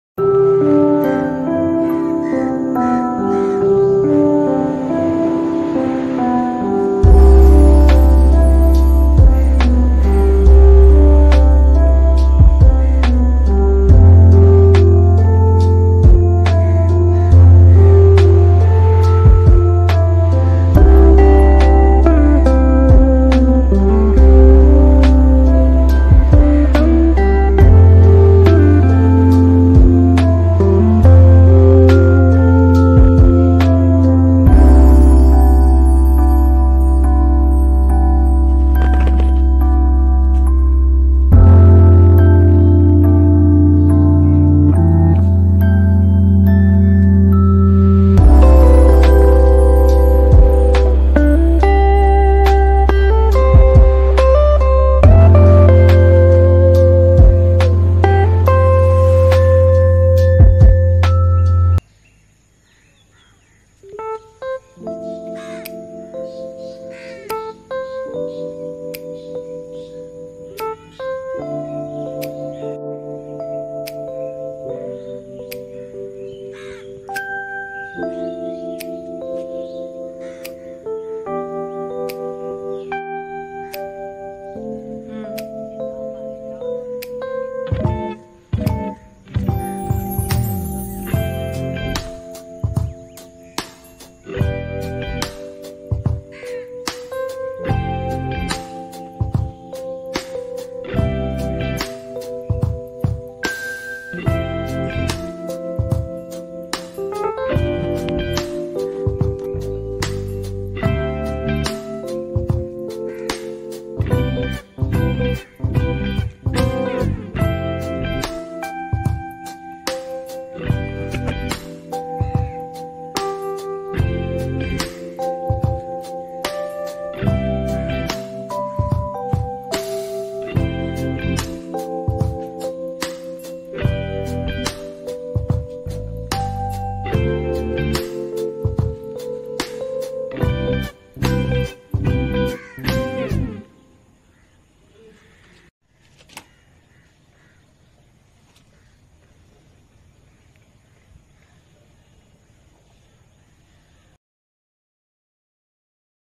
Green nature.....Keloka Residential Area ,my Colony
We live in a very greenery place, Alhadulillah...Share some moments with you guyss. I hope you all enjoy... Insha Allah.